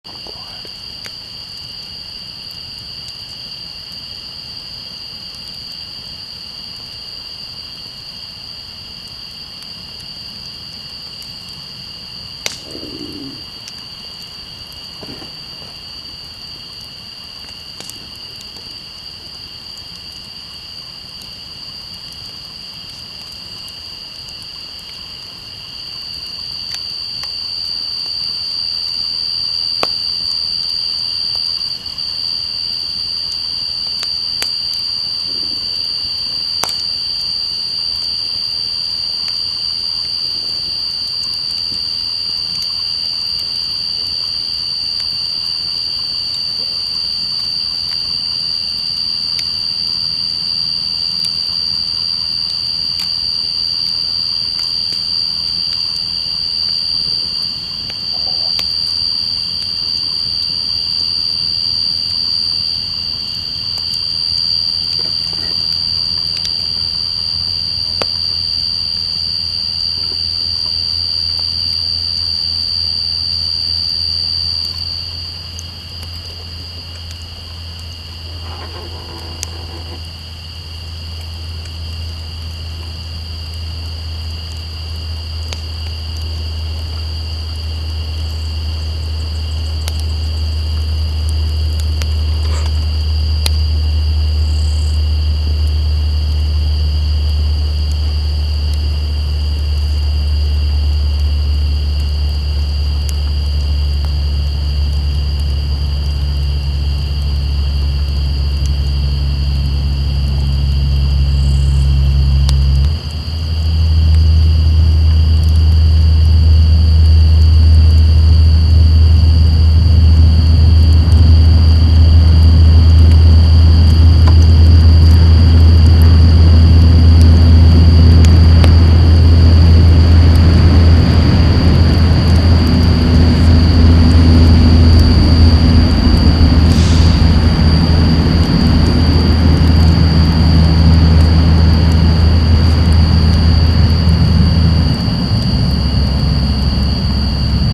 Campfire at night in outback Australia
A simple campfire at night with night sounds and a semi trailer thundering through the night..To be shared with film makers.